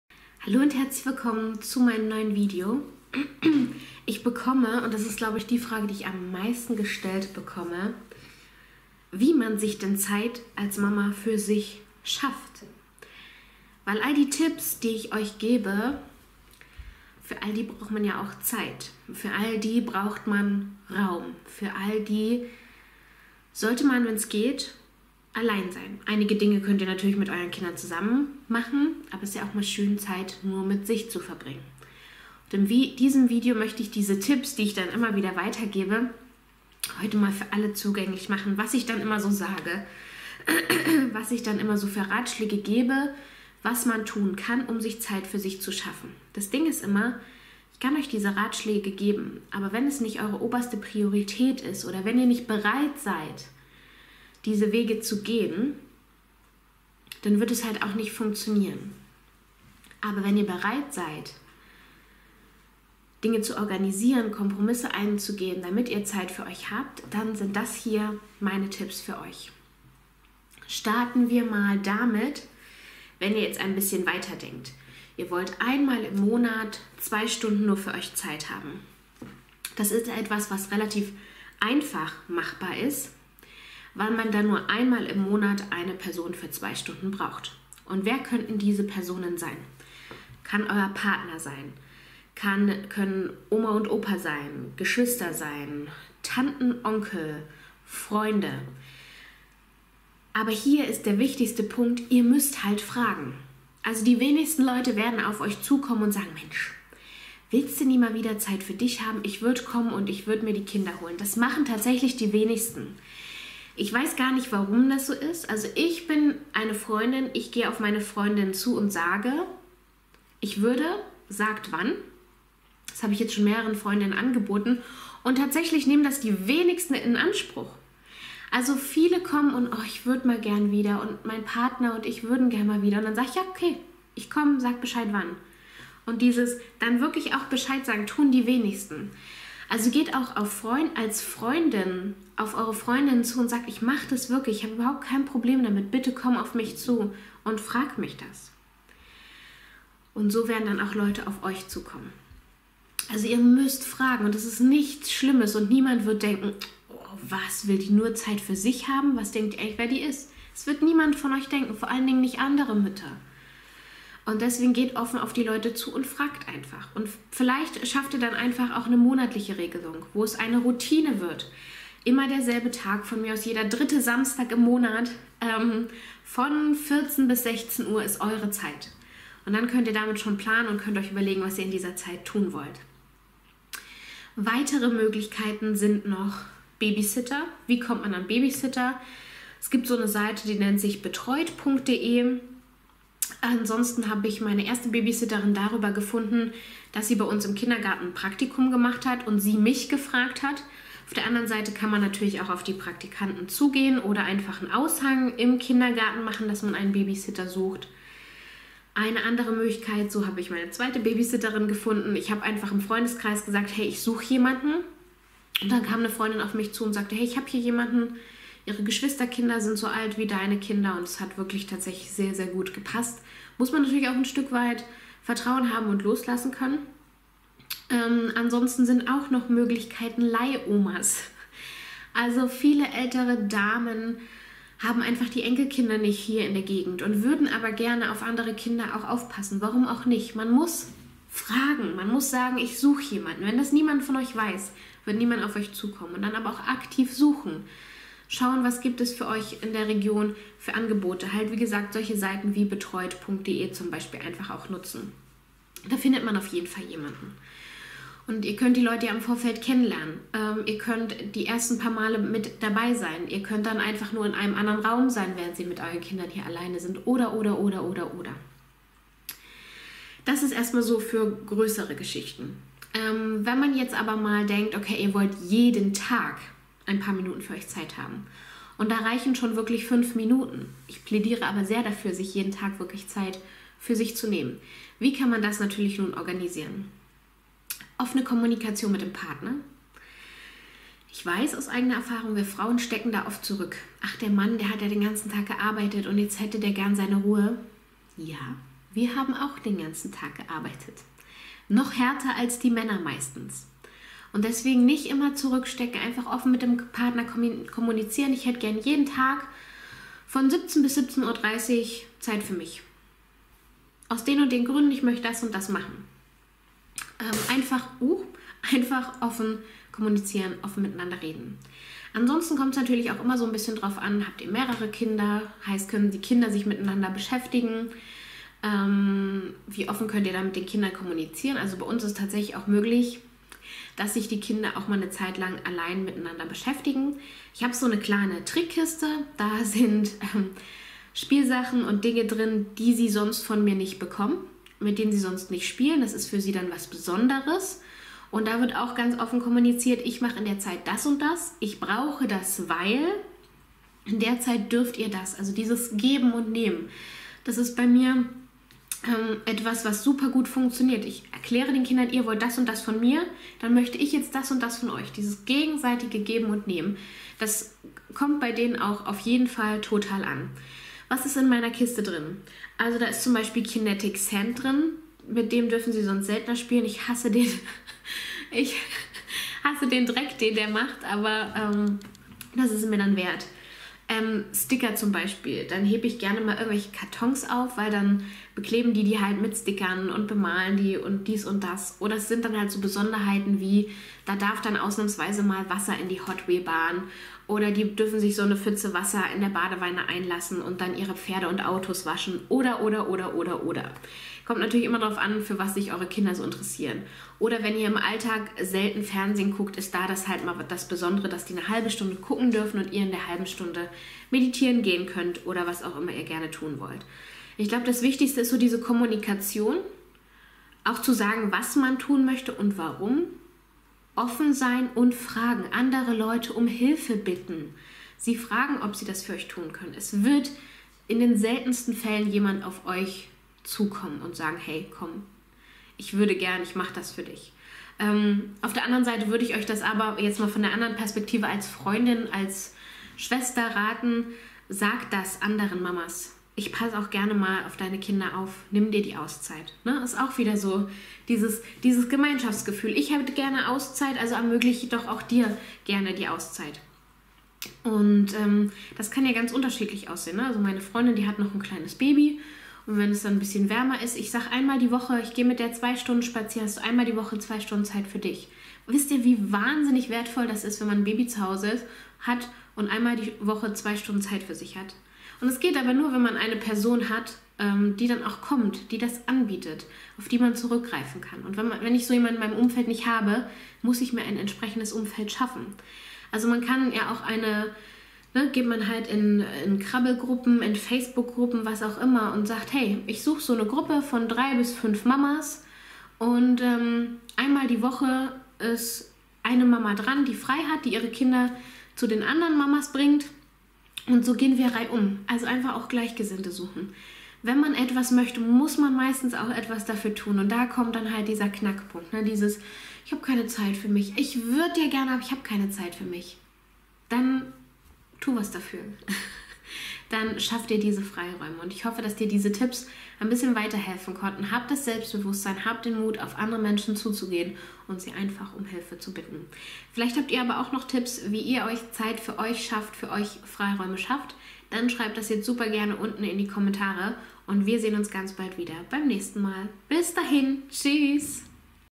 Als Mama Zeit für sich schaffen | Frau Farbenfroh
Wie kannst du dir als Mama Zeit für dich schaffen?

Wenn ihr uns etwas zuschicken möchtet, dann bitte an diese Adresse:

Julia Ressel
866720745
Packstation 113
02906 Niesky